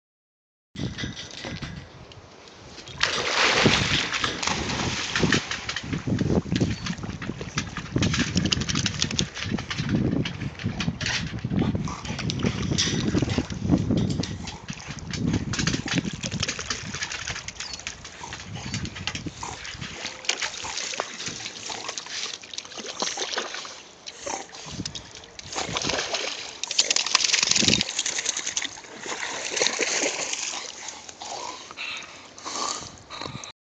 dog belly flop
star my springer spaniel diving off the jetty to retrieve a tennis ball